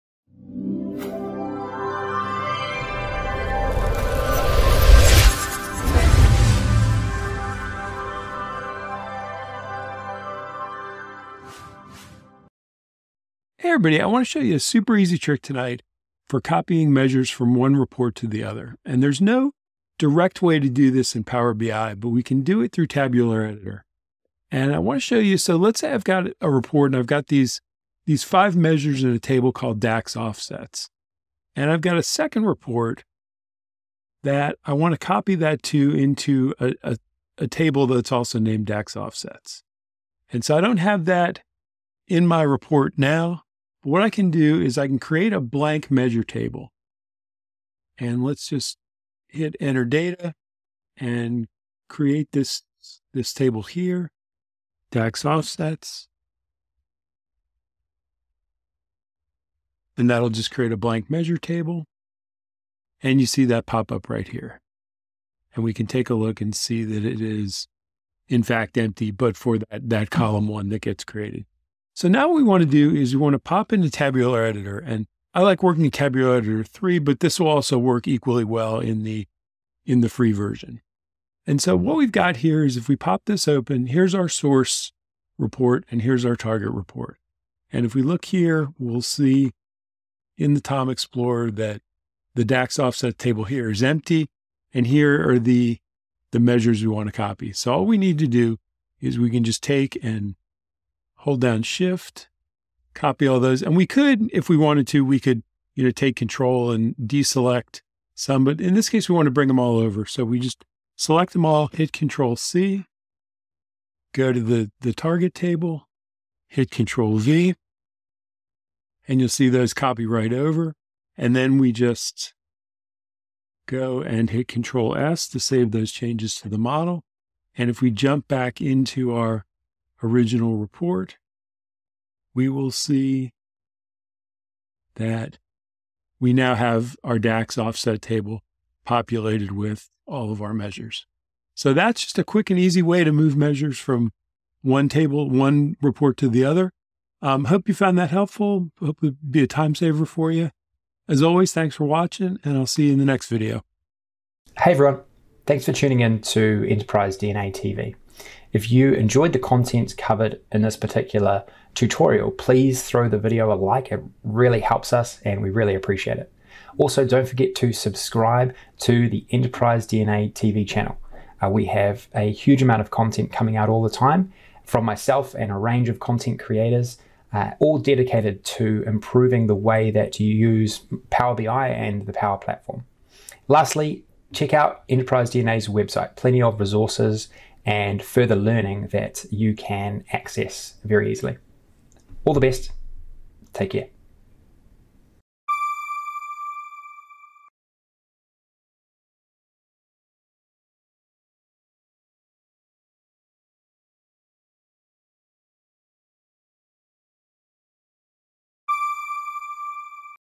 How To Bulk Copy Measures From Different Reports In Power BI
You’ll most probably encounter having to reuse the same measure when working with multiple Power BI reports. In this video, Brian shares a handy trick for copying measures from one report to another. There is no direct way to do this in Power BI but this is something you can achieve using the Tabular Editor.

Video Details
00:00 Introduction
00:27 Case scenario
00:45 DAX Offsets table
01:22 Copying measures